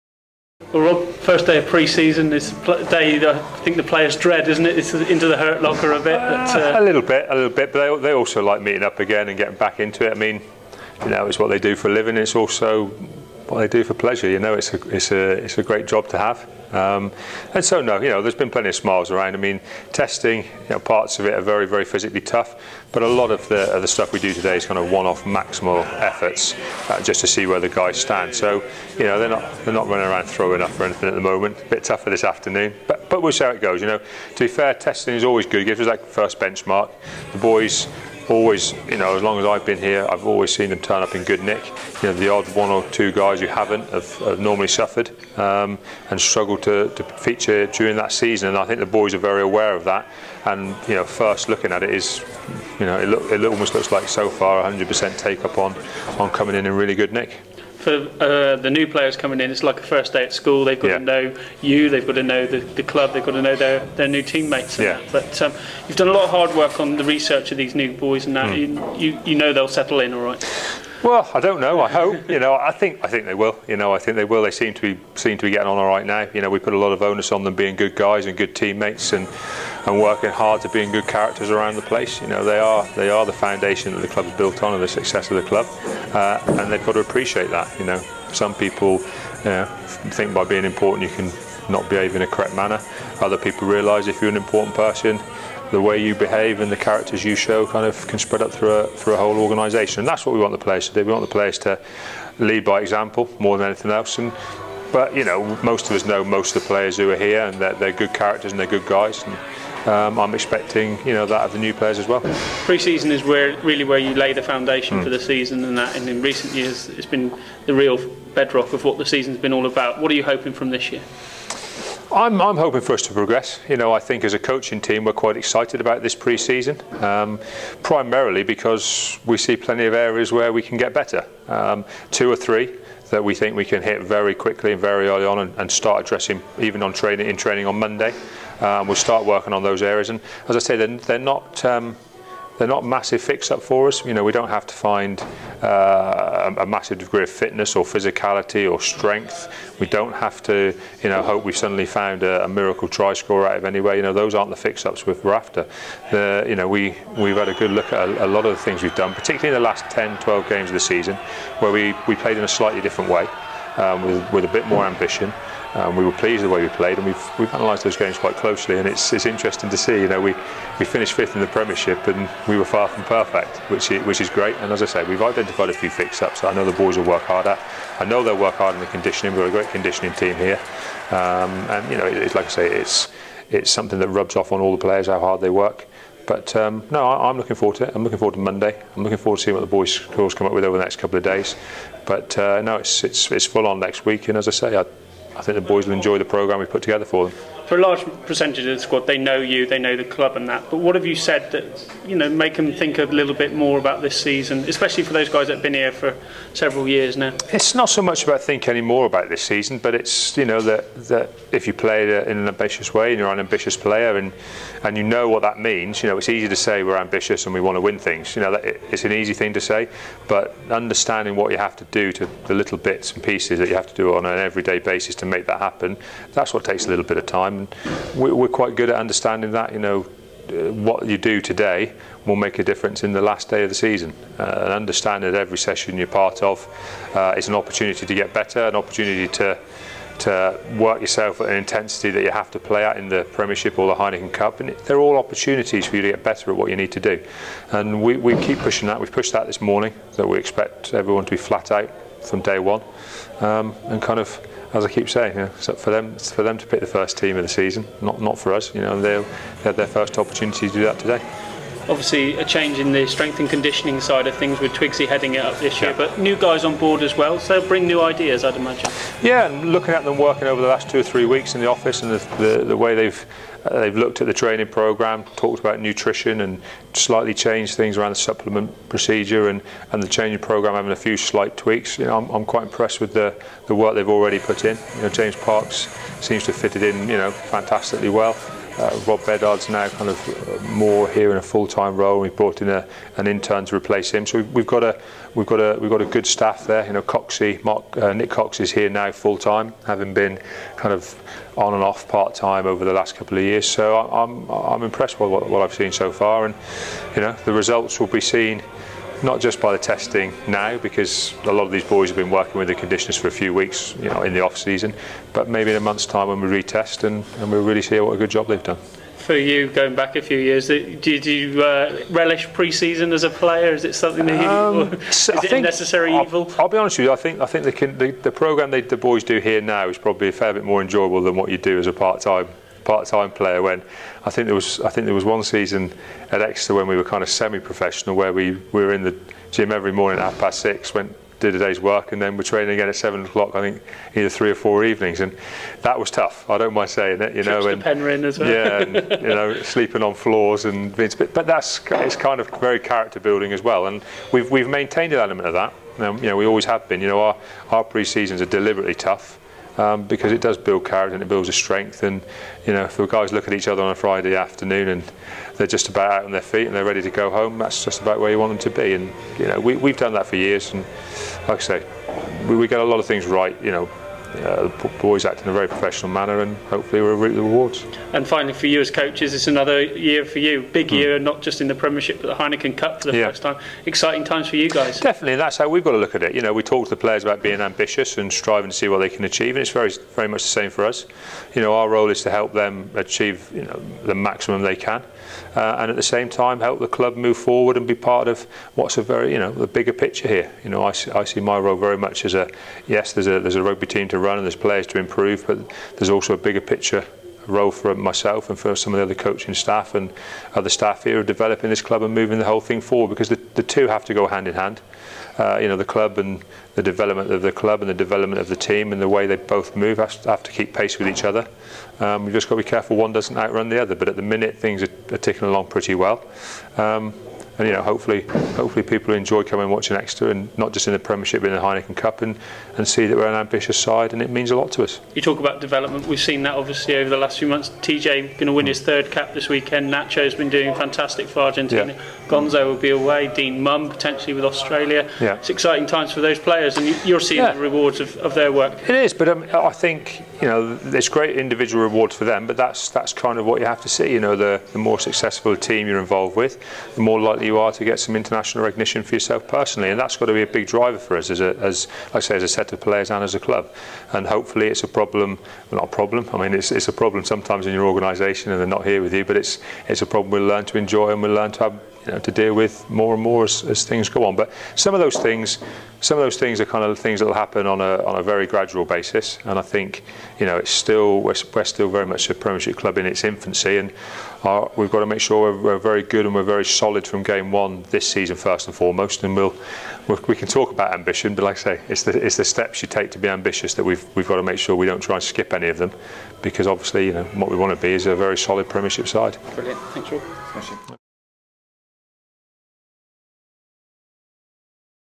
Chiefs TV Baxter ' First Day Back '
Chiefs TV - Rob Baxter talks during the ' first day back'  at Sandy Park whilst the players , old & new are put through their paces in the gym...